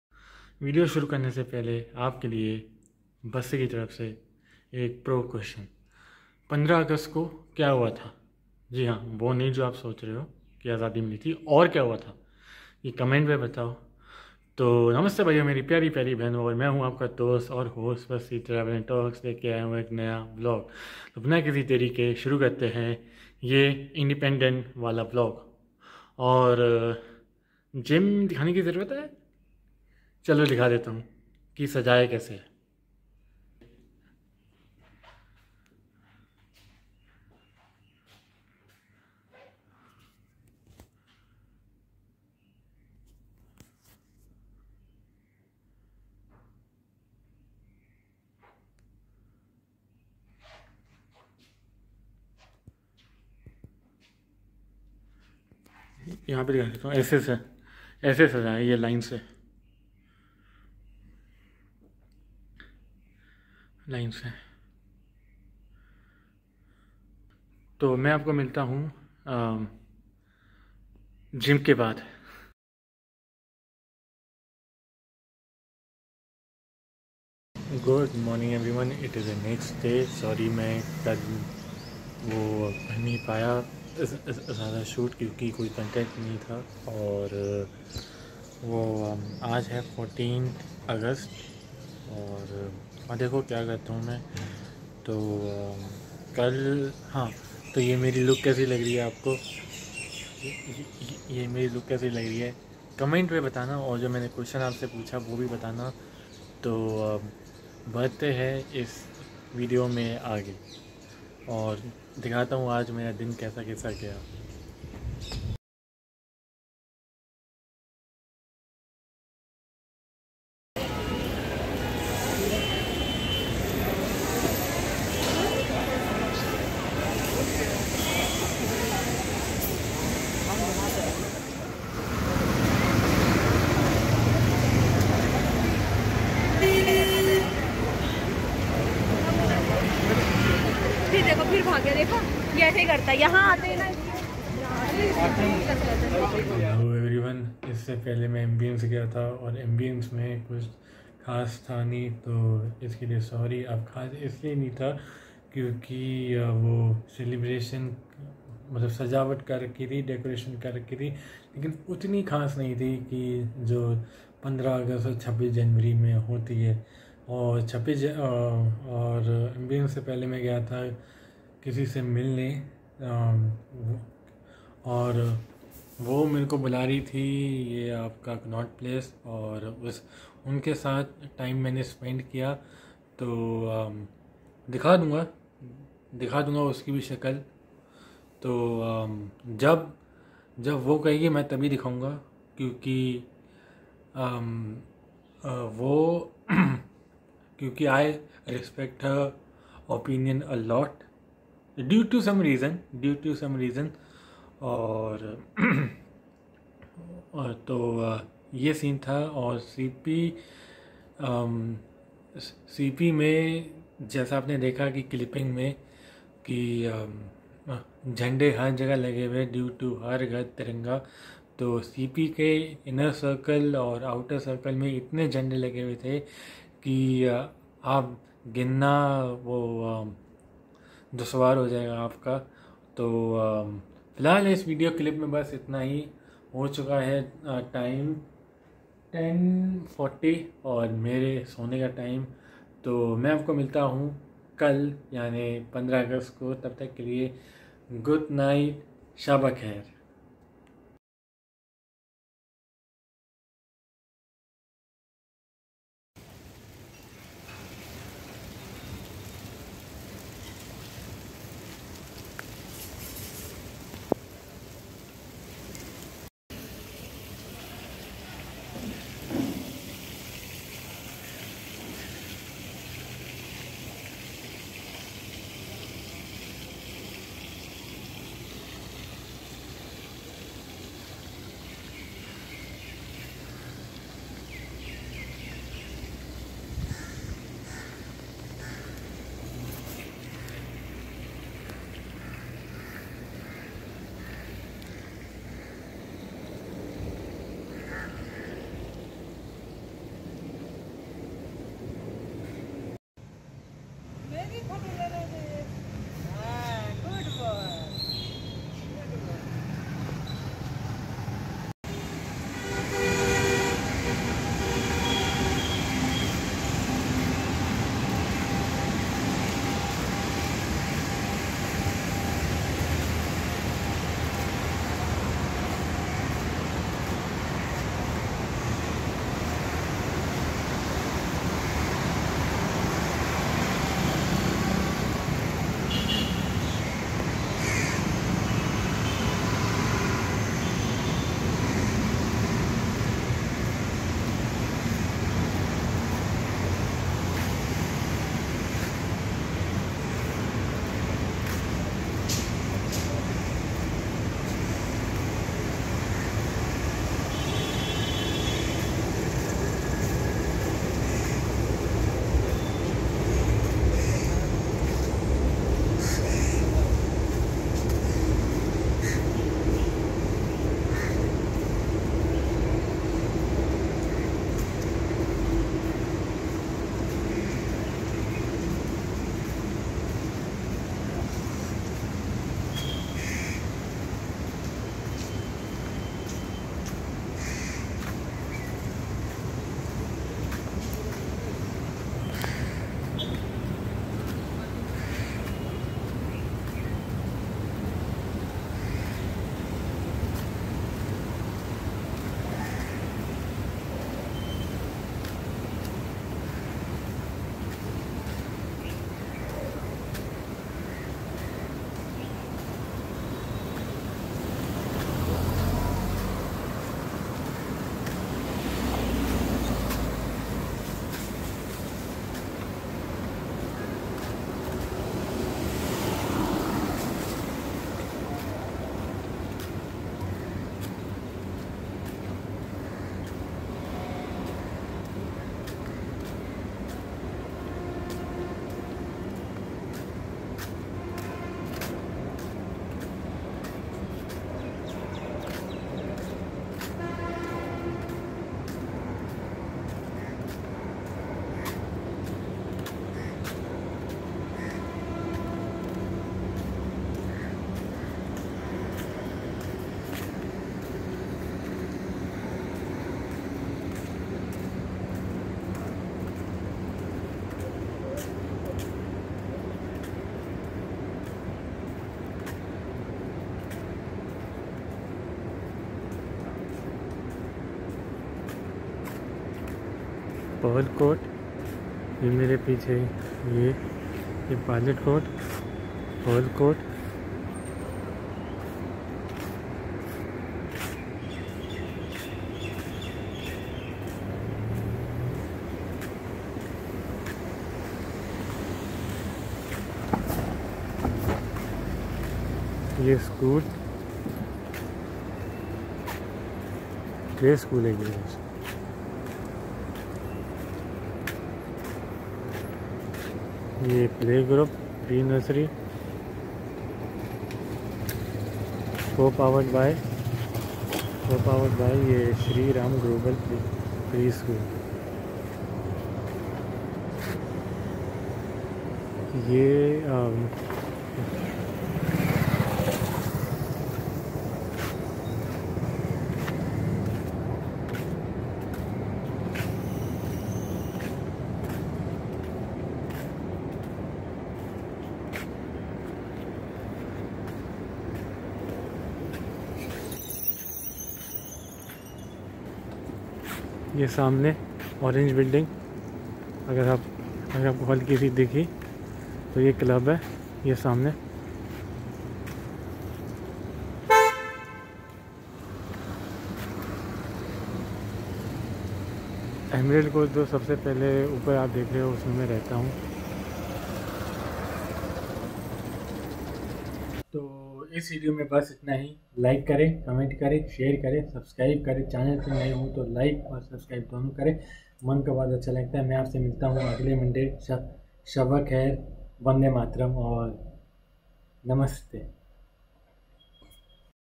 HAR GHAR TIRANGA IN ESSEL TOWER
'Har Ghar Tiranga' is a campaign under the aegis of Azadi Ka Amrit Mahotsav to encourage people to bring the Tiranga home and to hoist it to mark the 75th year of India's independence. The 'Har Ghar Tiranga' campaign ended on August 15.